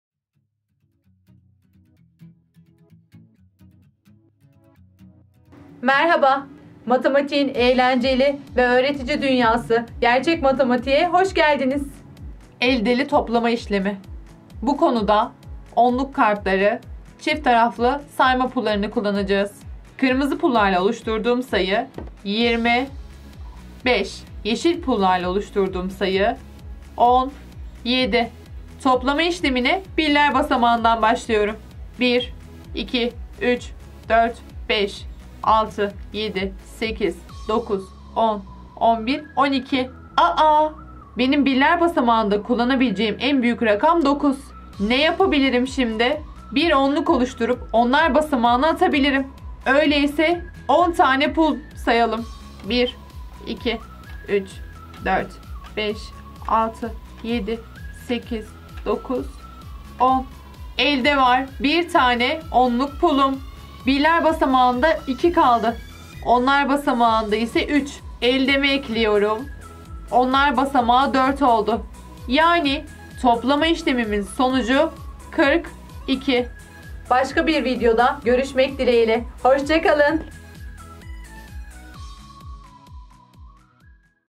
2. Sınıf Eldeli Toplama İşlemi
Eldeli Toplama İşlemi ile ilgili Gerçek Matematik materyallerinin nasıl kullanılabileceğini gösteren videodur.